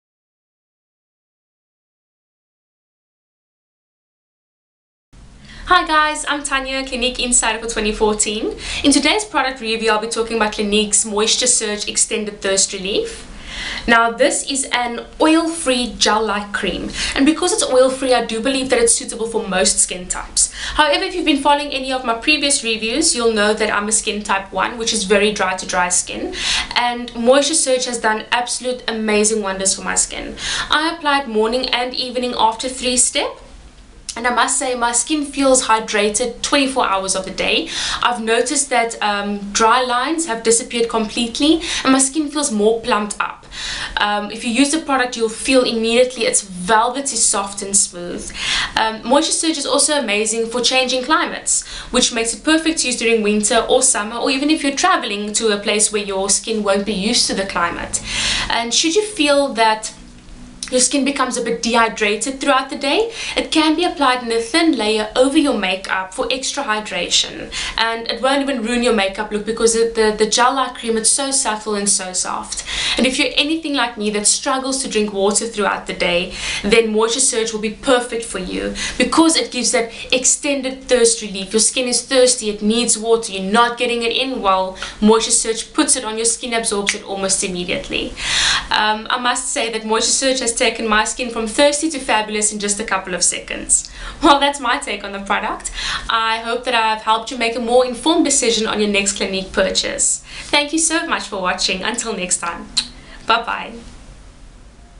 Moisture Surge™ Extended Thirst Relief Review by Clinique Insider Tanya
Find out what Tanya loved about using the Clinique Moisture Surge Extended Thirst Relief

Skin Types: Works for all skin types.
Lightweight cream-gel plumps skin with 24-hour hydration. Oil-free.
Its unique texture must be experienced to be believed.